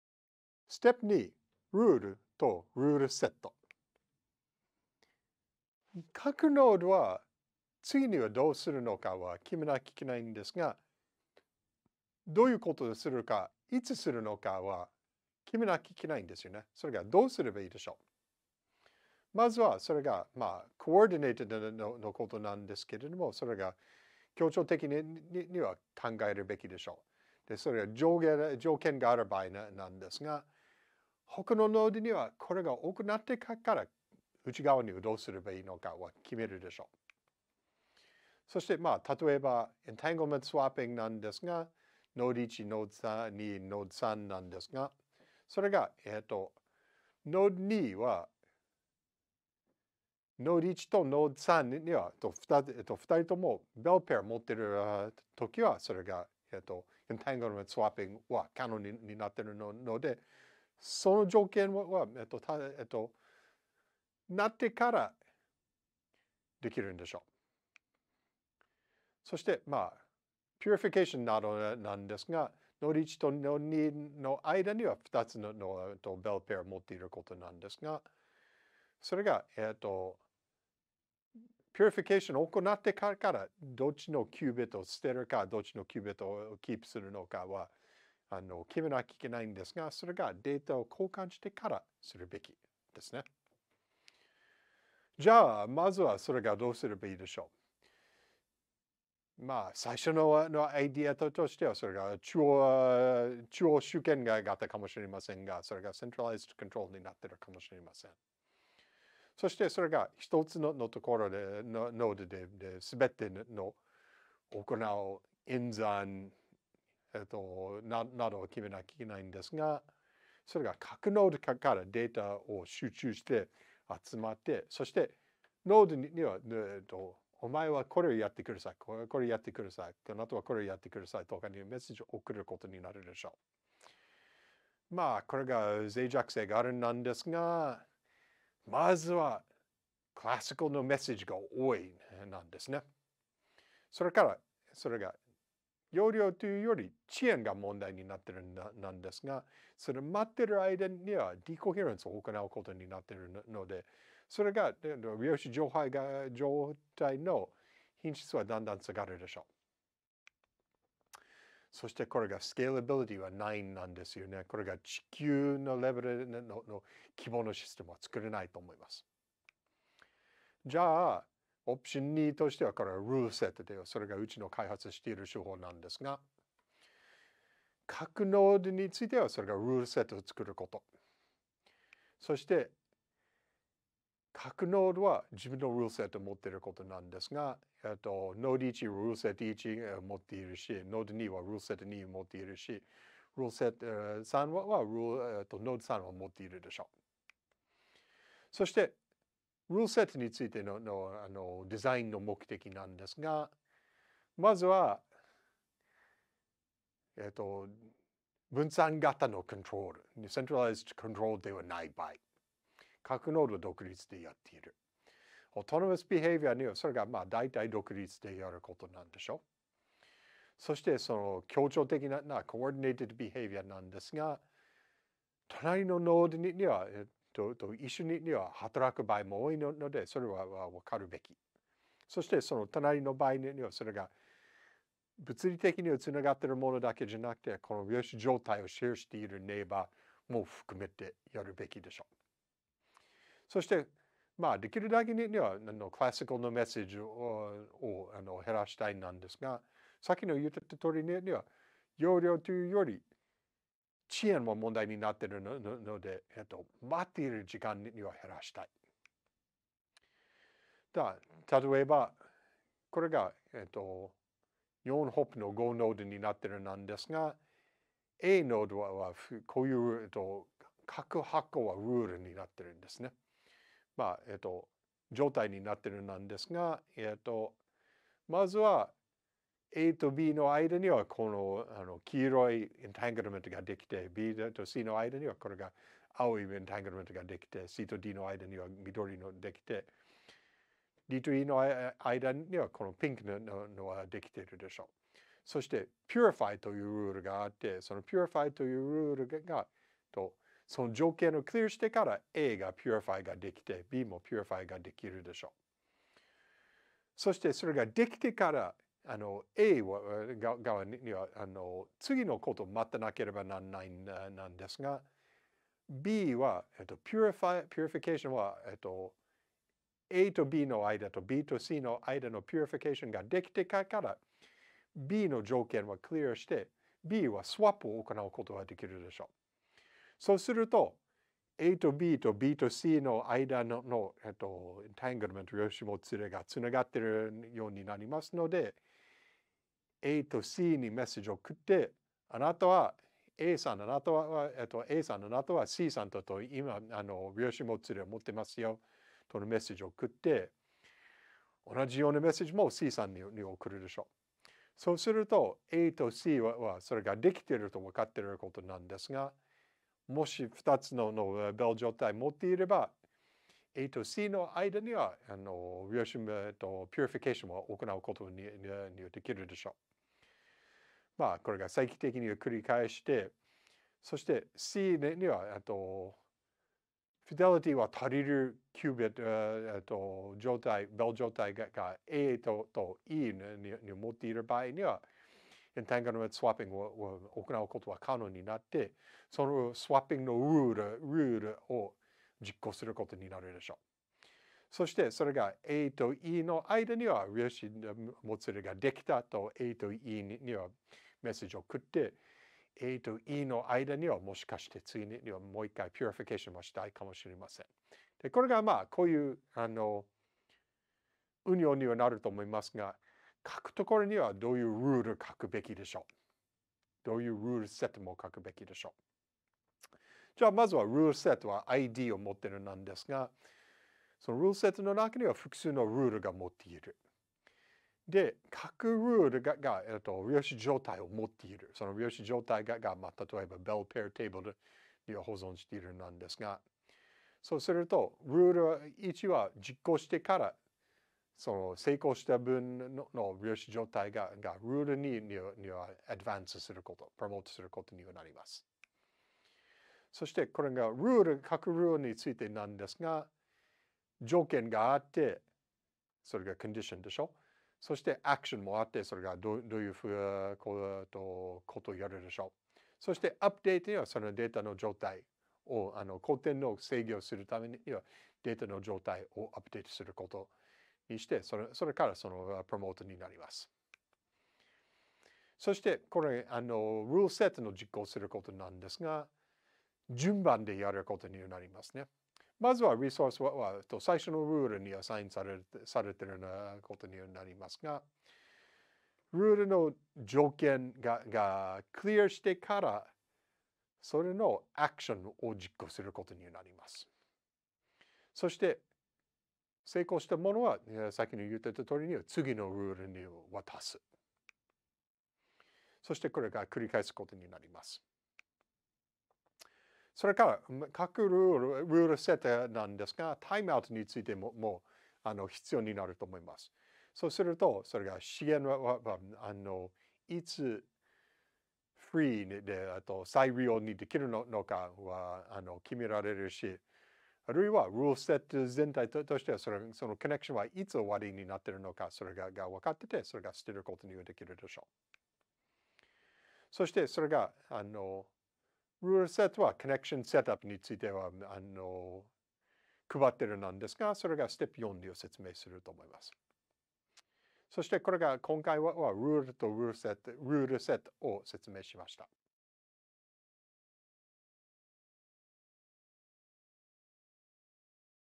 8-2 ルールとルールセット
レッソン　8　接続
ステップ　2：　ルールとルールセット
ルールとルールセットの概念を導入することで、個々のノードがどのようなアクションをいつ実行するかをどのように知るかについて考えます。

本ビデオは「量子インターネット」モジュールの一部です。